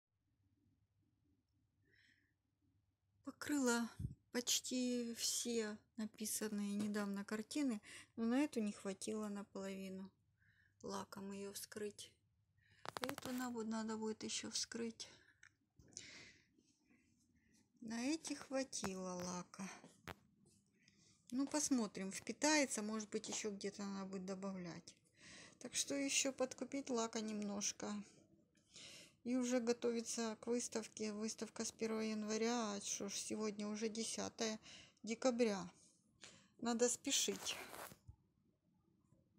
Готовлюсь к персональной выставке. 10.12.20
Начала интенсивную подготовку к персональной  выставке. Нужно еще несколько новых картин написать, потому что нетерпится претворить новые идеи. И нужно подготовить написанные  картины, т.е. покрыть их лаком, подписать,  нужно сложить их в коробки и отправить в галерею.